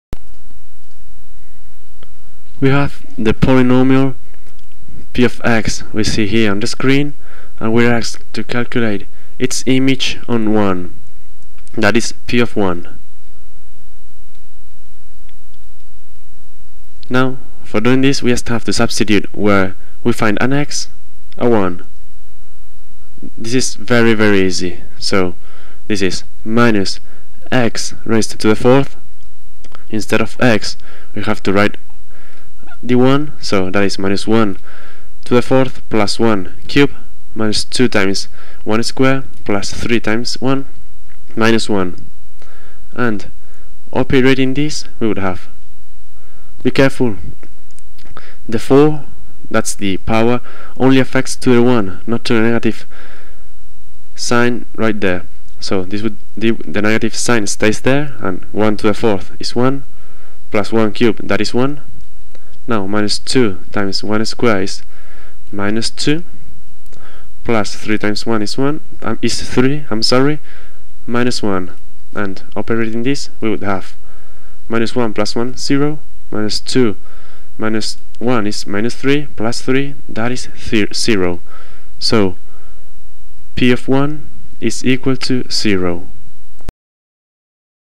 Evaluating a polynomial
In this video we evaluate a polynomial in a number.